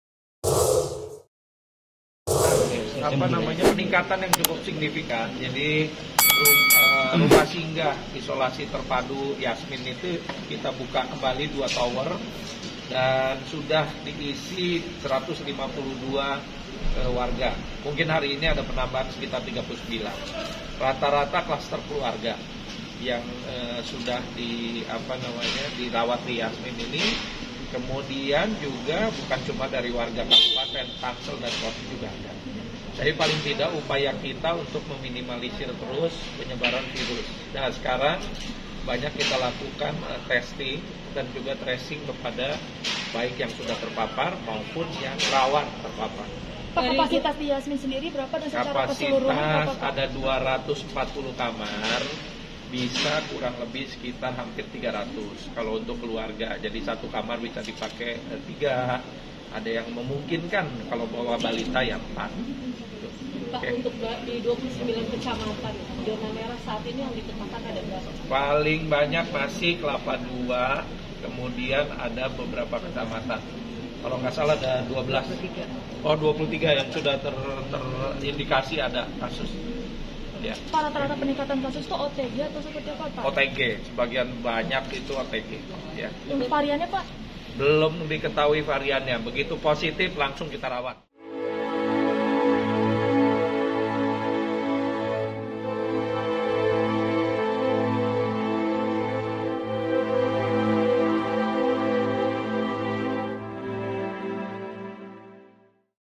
Kasus Covid-19 Meningkat Signifikan Dalam dua Pekan di Kabupaten Tangerang
Pemkab Tangerang bersama Polda Banten dan TNI terus mengantisipasi kembali merebaknya kasus Cocid-19 yang dalam dua pekan meningkat signifikan. Di antaranya dengan menyiapkan fasilitas isolasi bagi warga yang terpapar virus tersebut.

Bahkan untuk memenuhi kebutuhan isolasi warga, dua tower Hotel Yasmin disiapkan sebagai tempat karantina warga. Bupati Tangerang, Ahmed Zaki Iskandar mengatakan penambahan kasus Covid-19 didominasi klaster keluarga dengan kategori Orang Tanpa Gejala atau OTG.